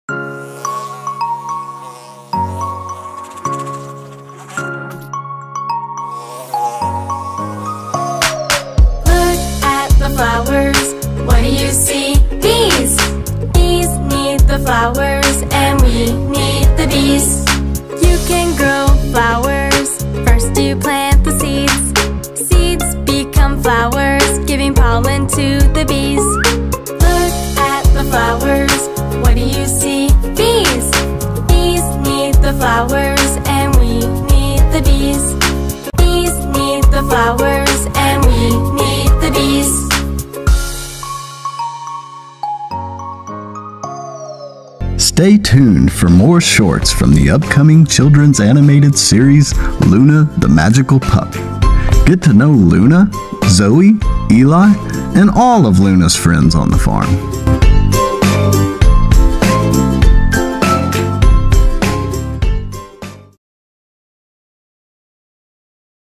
Thankful for the Bees! 🐝
Hi I'm Luna the Magical Pup! Thank you for visiting my YouTube Channel!! Today I wanted to share a video featuring our bee friends!! 🐝🐝 🐝 Bees sure are a Blessing and I am thankful for them!!